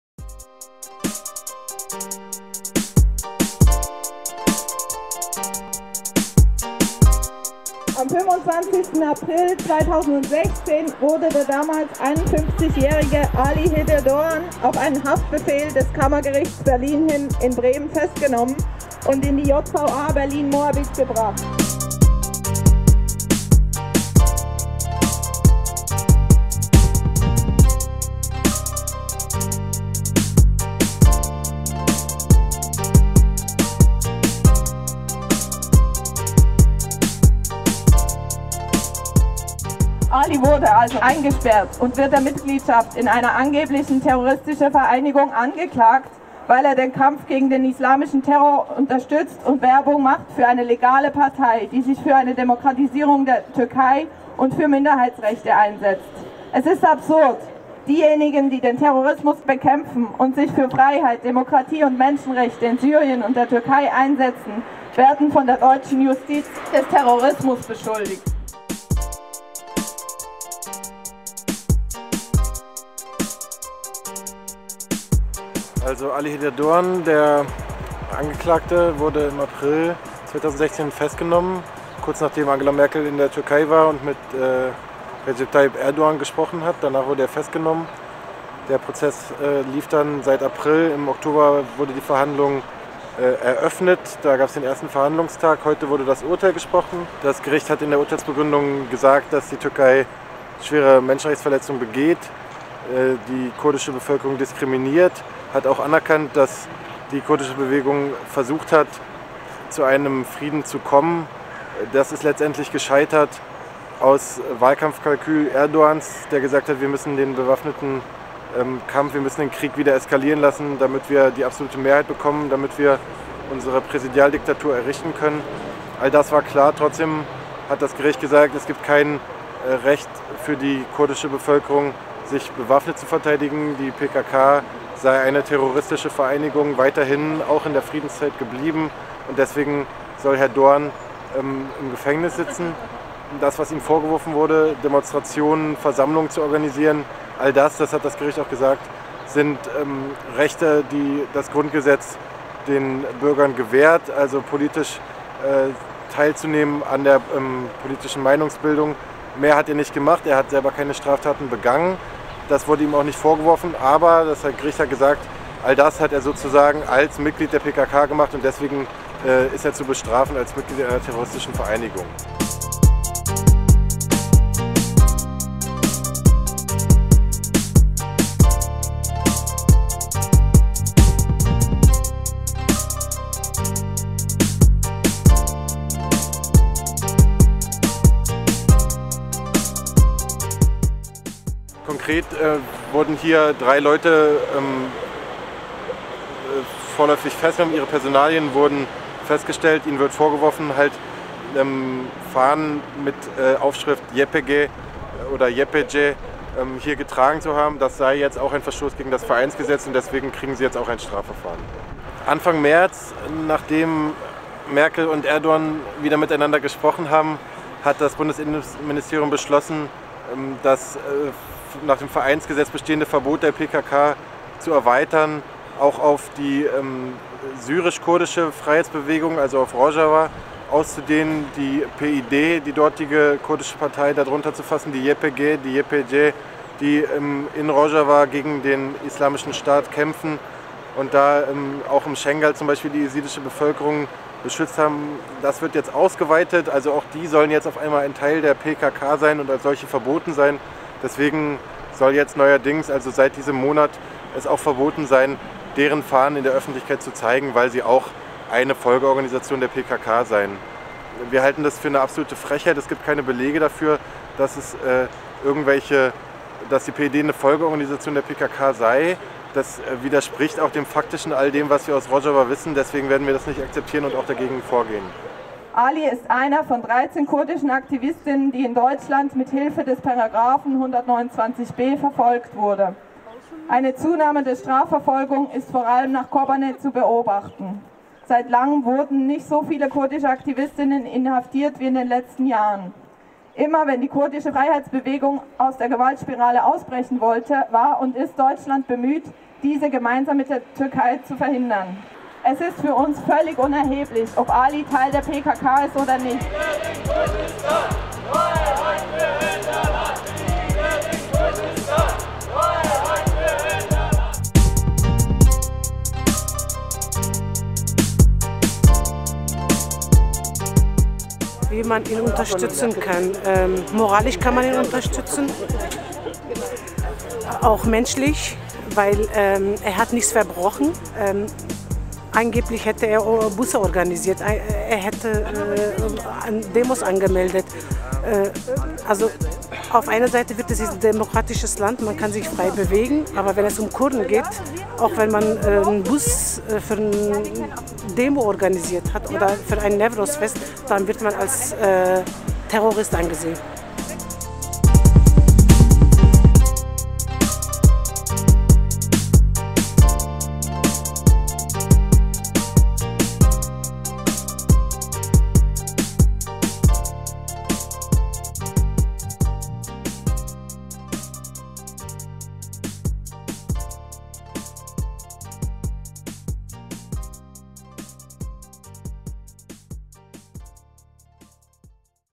Kundgebung & Prozess gegen Ali Hıdır Doğan - Videobericht 17.03.2017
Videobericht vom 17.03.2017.
Ali wurde heute nach Paragraf 129b verurteilt und inst nun im Knast. Wir sprachen mit Freund*innen vor dem Gericht und seinem Anwalt.